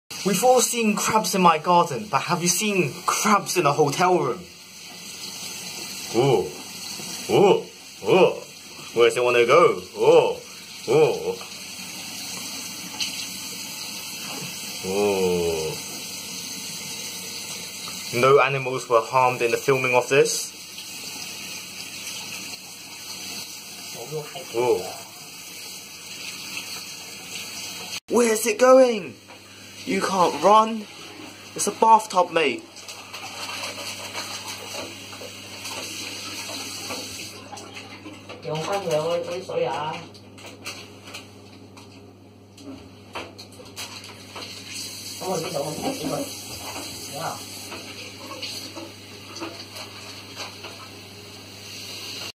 Crabs in Hotel Room
We've all seen crabs in my garden but have you seen crabs in hotel room.
If you have any questions or things to say to me leave them in the comment section down below.